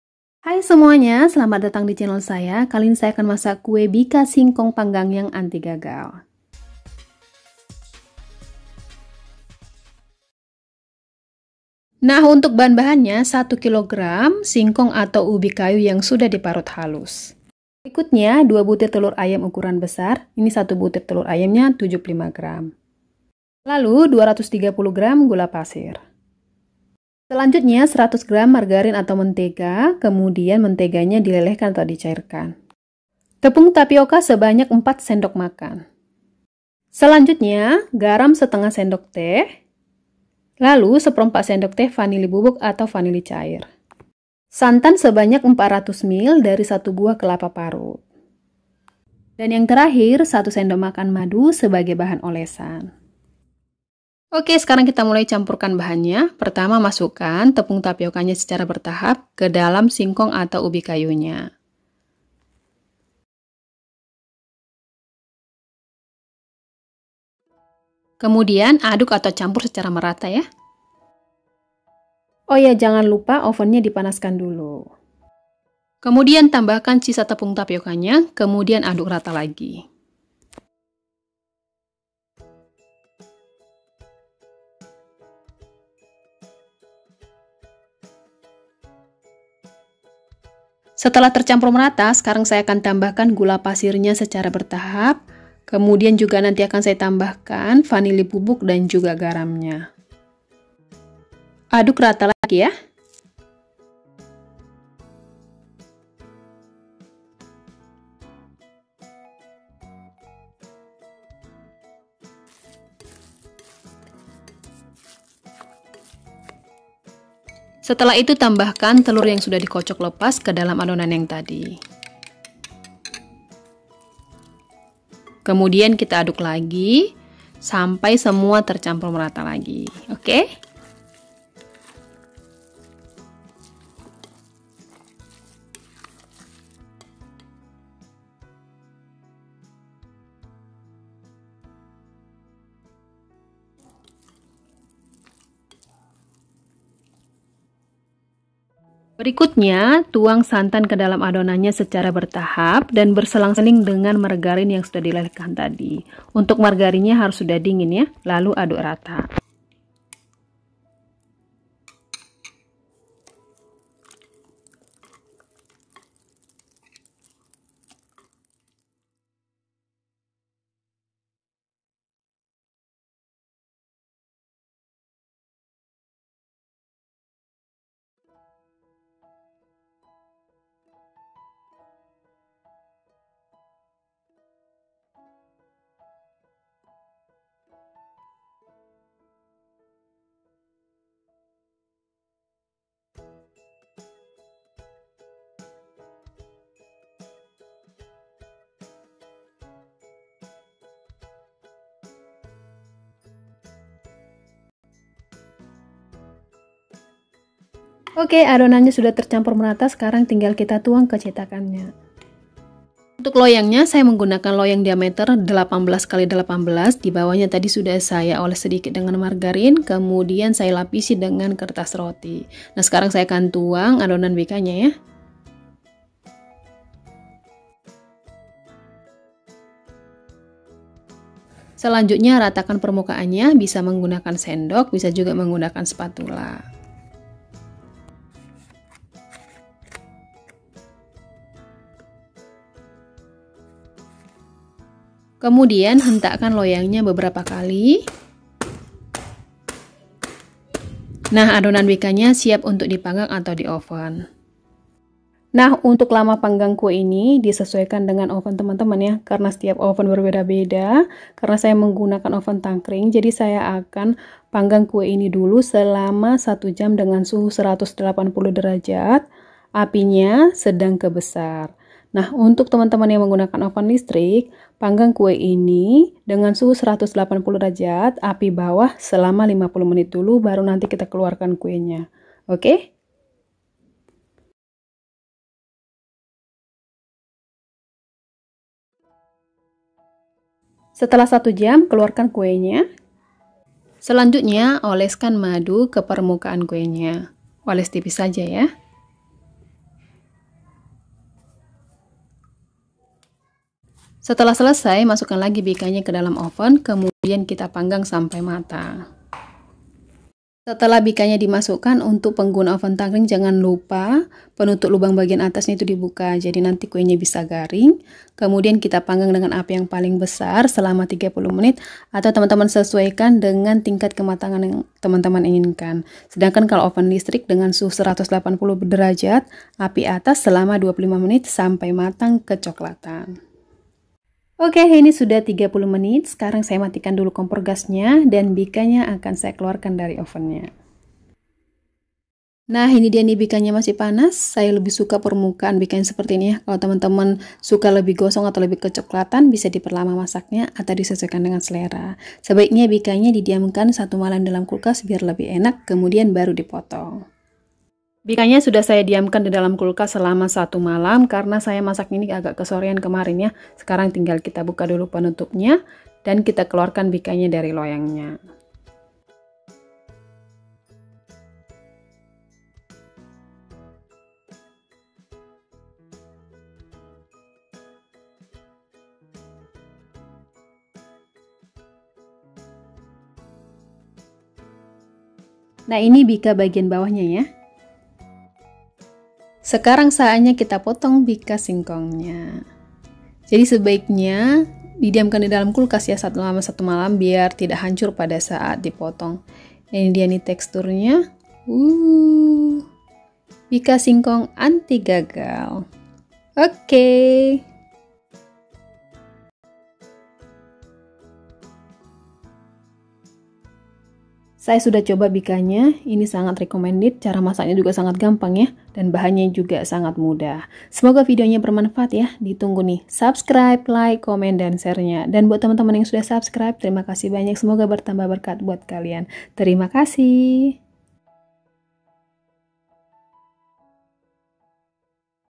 Singkong dimasak jadi Bika, wahh rasanya mantul banget|| Bahannya mudah dan anti gagal
Bika singkong atau ubi kayu panggang anti gagal ini sangat mudah utk dipraktekkan di rumah. Rasanya benar2 mantul, bahannya mudah, masaknya jg gampang. Bisa disajikan sebagai cemilan atau bahan jualan.
Berikut bahan2nya:
Singkong parut 1 kg
Santan 400 ml
Margarin 100 gr, lelehkan
Tepung tapioka 4 sdm
Vanili 1/4 sdt
Garam 1/2 sdt
Madu 1 sdm

Selamat mencoba

Terima kasih